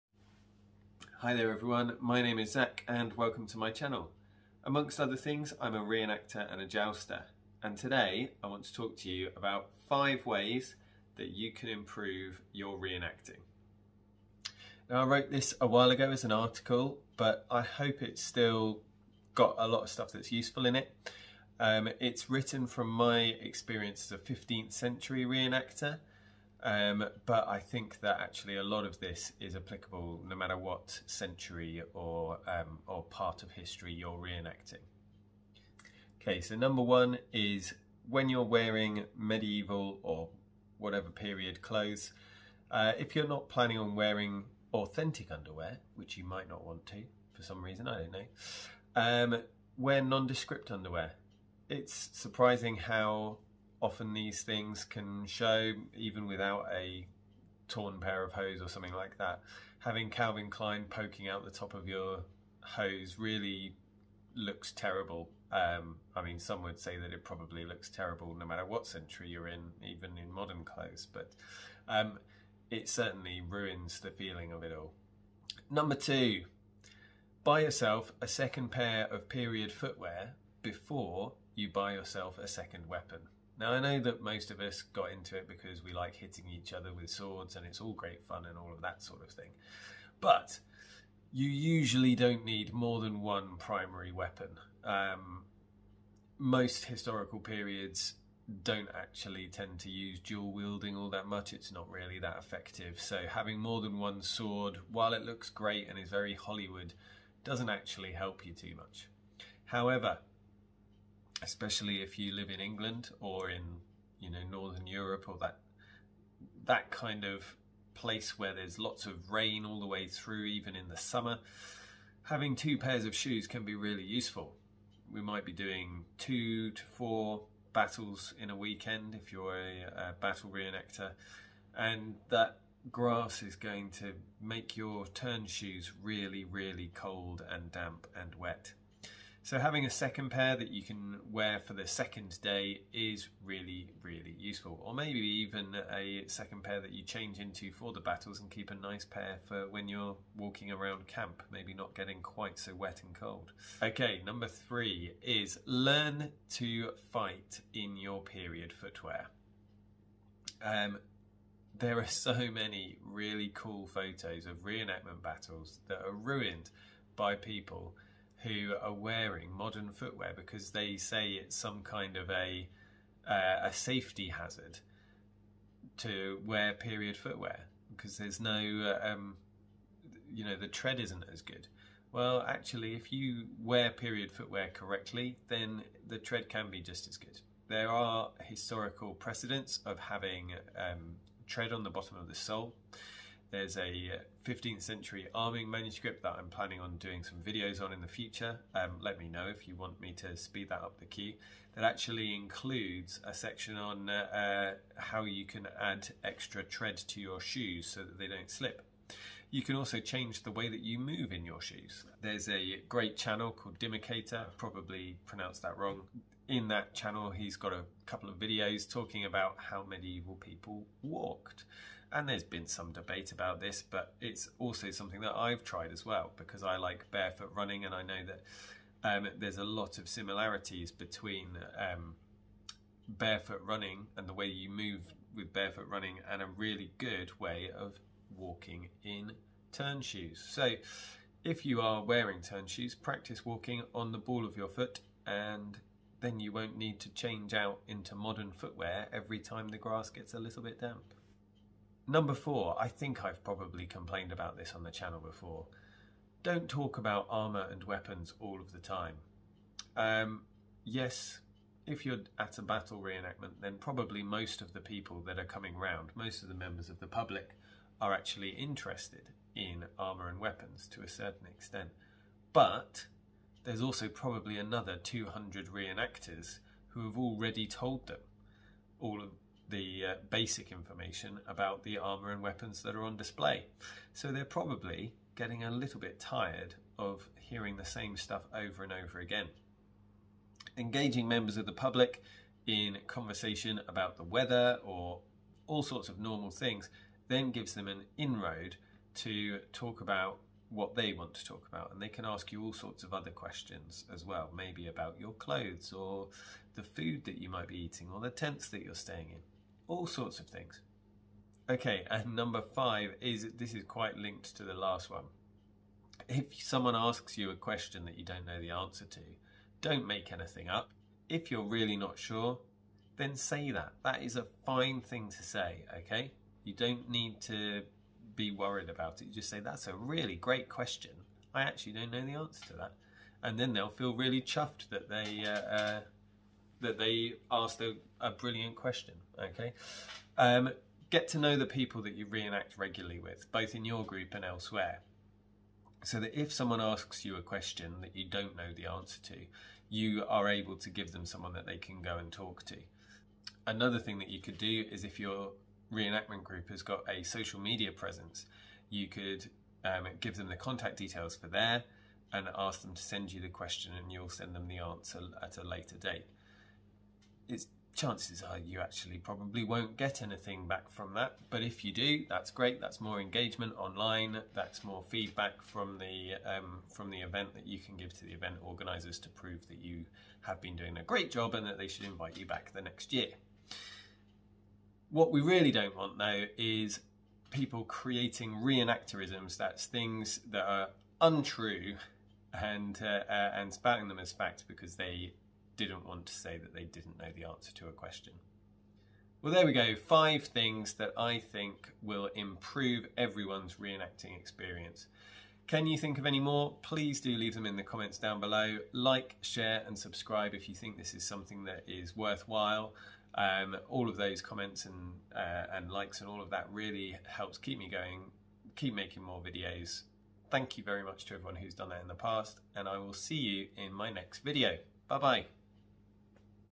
5 Ways to Improve Your Re-enacting
This week I decided to share a post I wrote a really long time ago about how to be a better reenactor. I think a lot of the advice still stands up.

What advice would you give to someone who wants to improve their re-enacting?